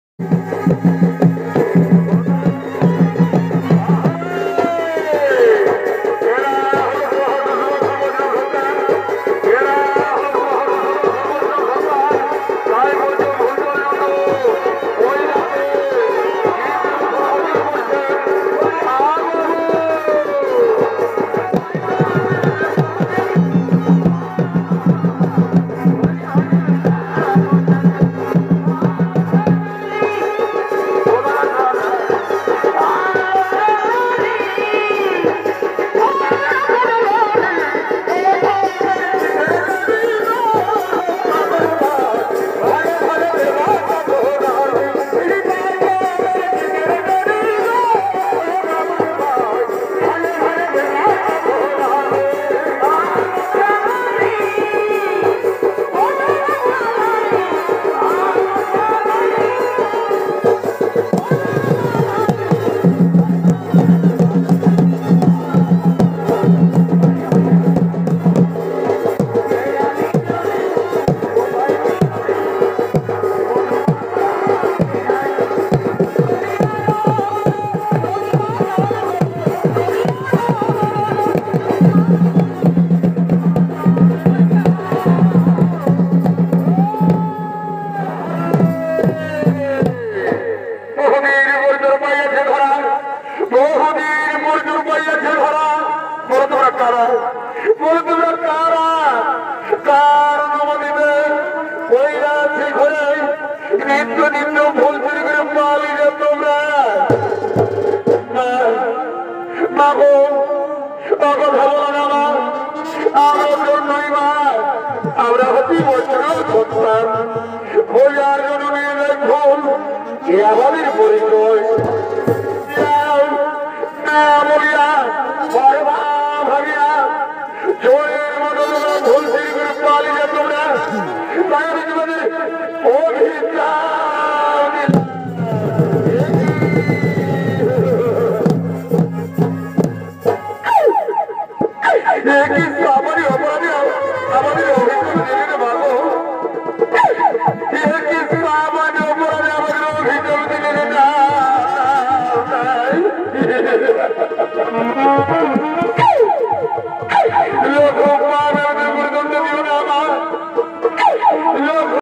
এই নাচ প্রথম বার বিরাট দিল | New Cho Nach | Muruli Singh Sardar Cho Nach 2023 | Chonach Muruli Singh
Muruli Singh Sardar new purulia cho nach